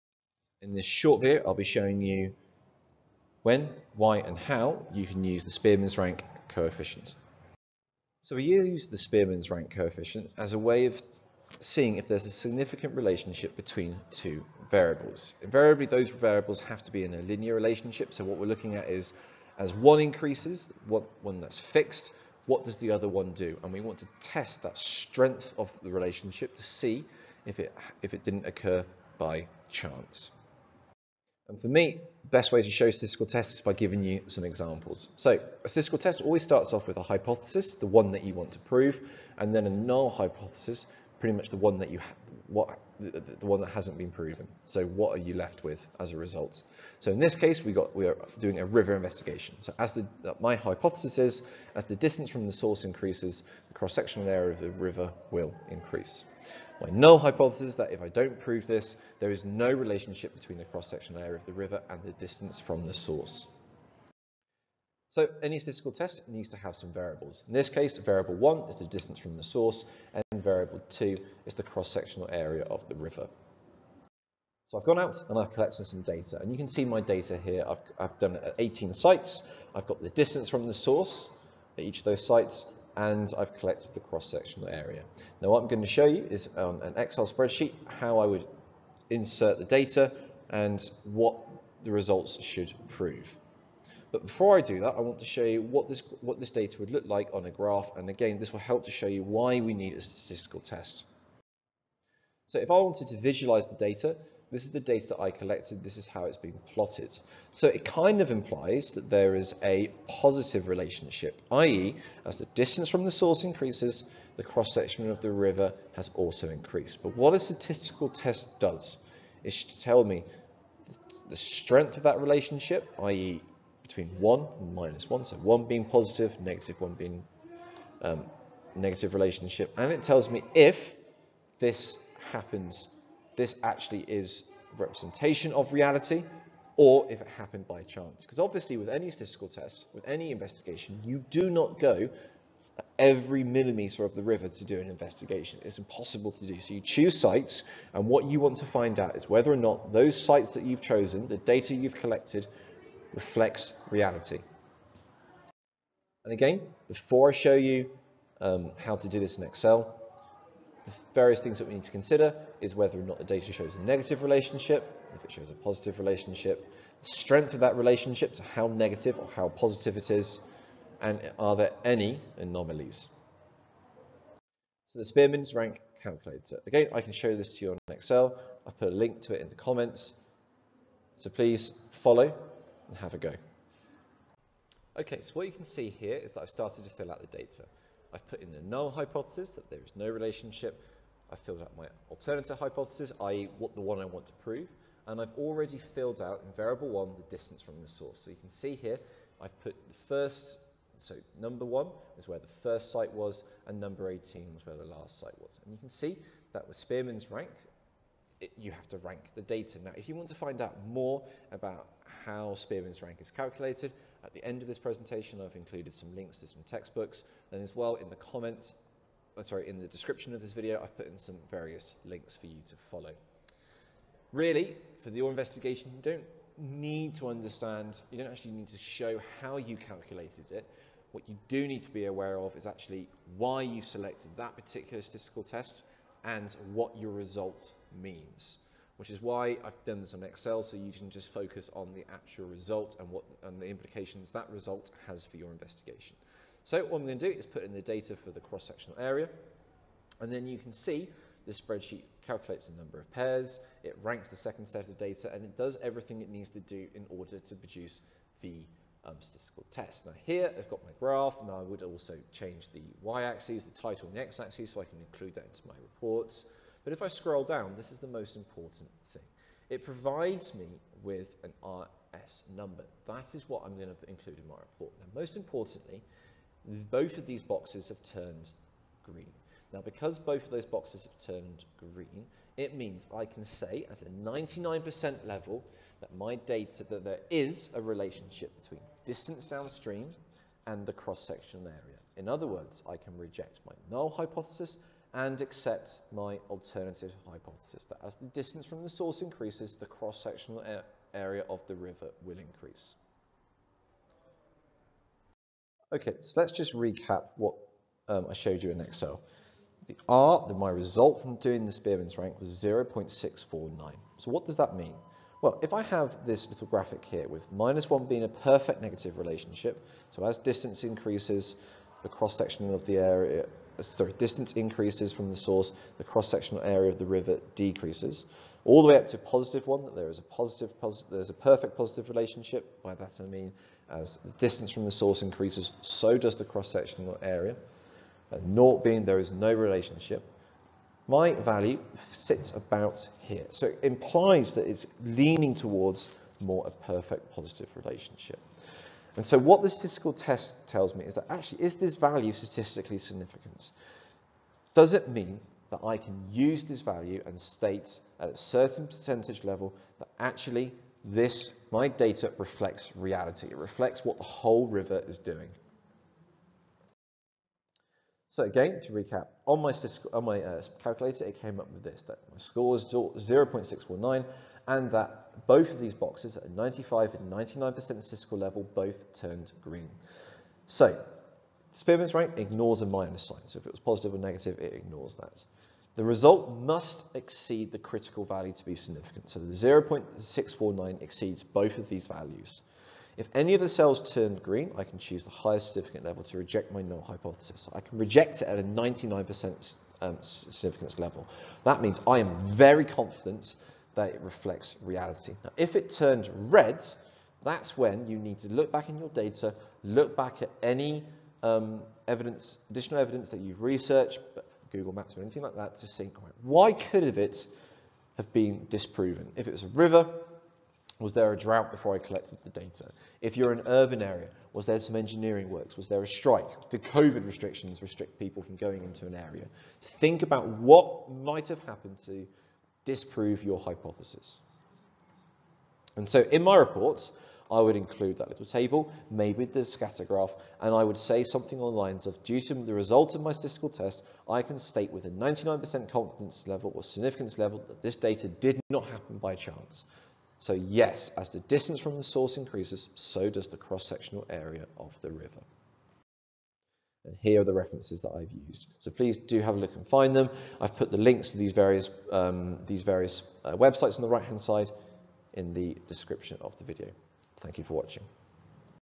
Spearman's Rank Explained
How to use the Spearman's rank statistical test in your investigation.

Here are some links that explain to you the test in a bit more detail:

Let me know if you would like the calculator.